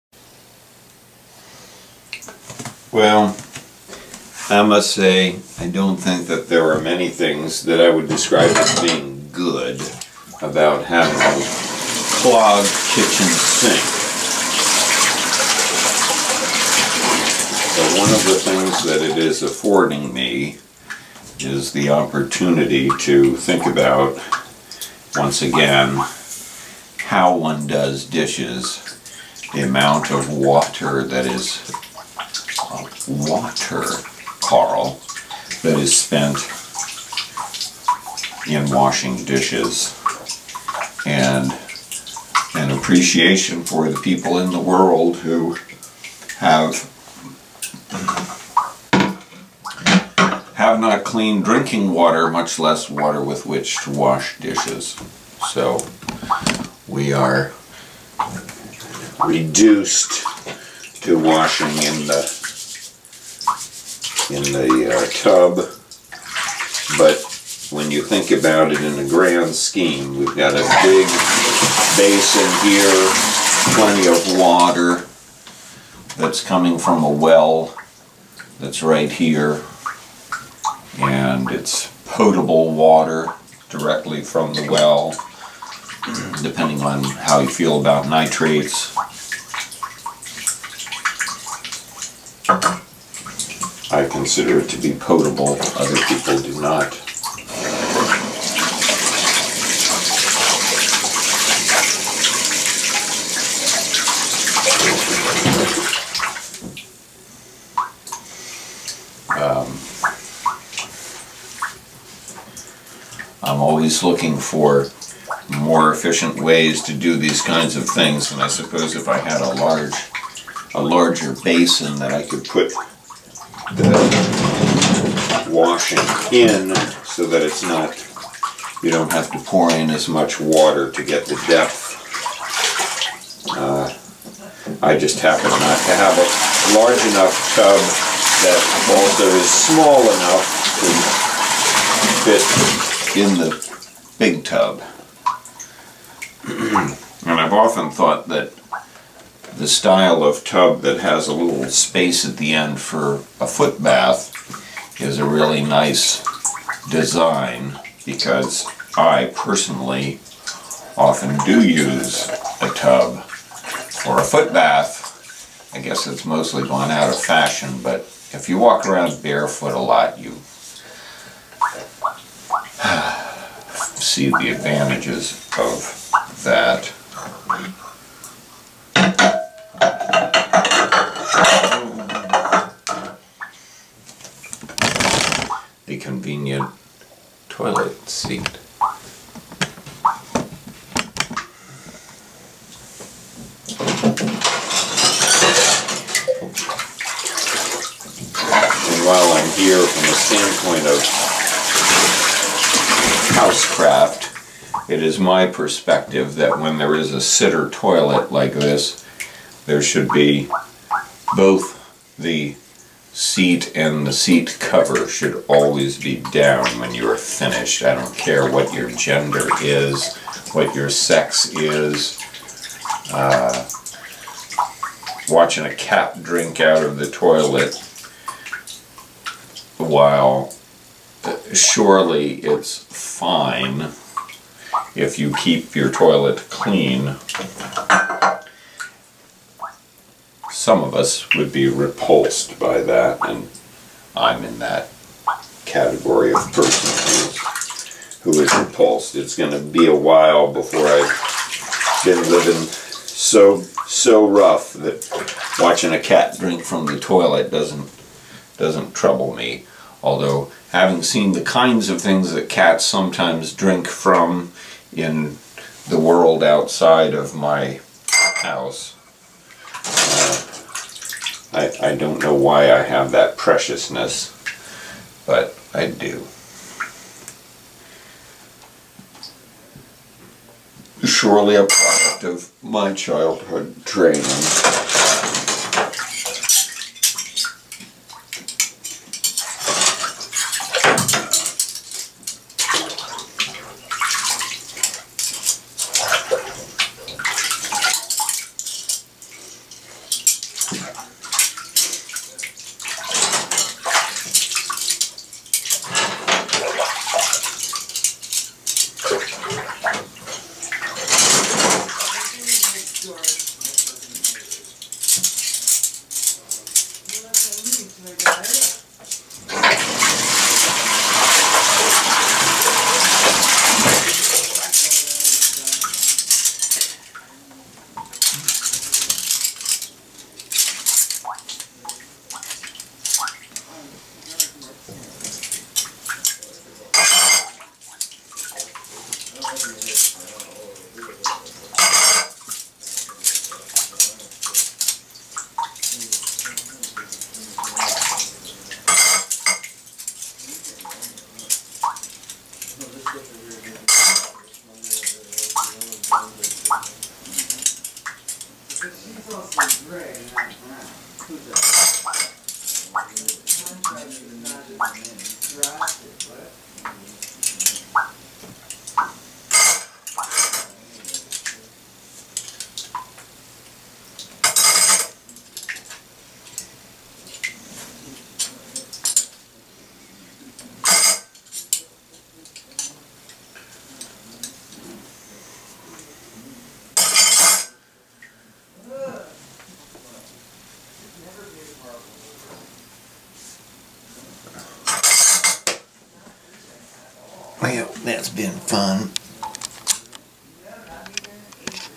clogged kitchen sink so wash dishes in the tub BDMV Clip11
July 15, 2019 Slackwater and really slack water. When new-to-us sink was installed, somehow the dishwashing machine, which had never been quite right, began a slow but inevitable decline and some combination of factors led to the dishwashing machine failing at about the same time and perhaps because of a clog in the drain from the sink. When we returned from Hagerstown, we found the sink almost unusable; thinking the problem would quickly be solved, I moved to washing in the tub we generally reserve for bathing.
Apologies to viewers in communities were water is not so readily available.